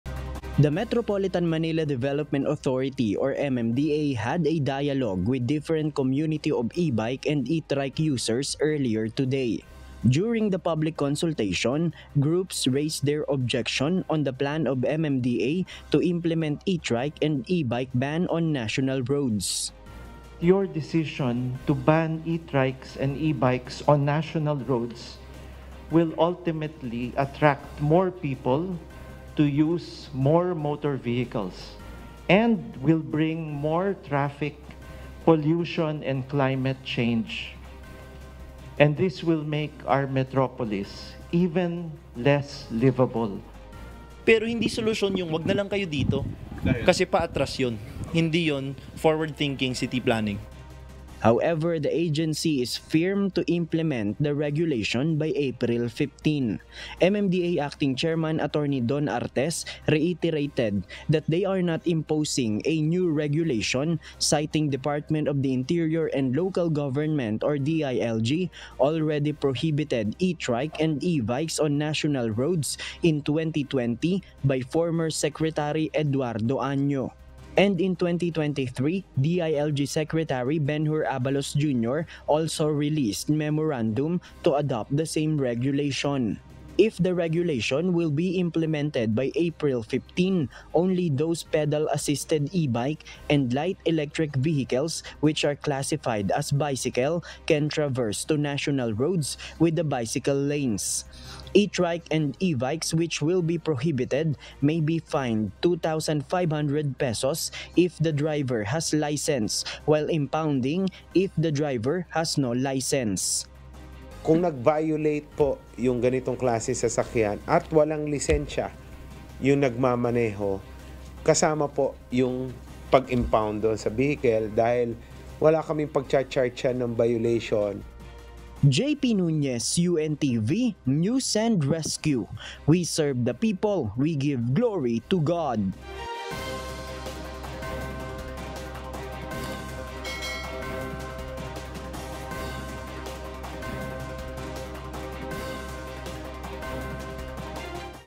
MMDA firm to implement e-trike, e-bike ban on national roads despite criticisms
The Metropolitan Manila Development Authority has planned to implement ban of e-trikes and e-bikes on national roads despite criticism from stakeholders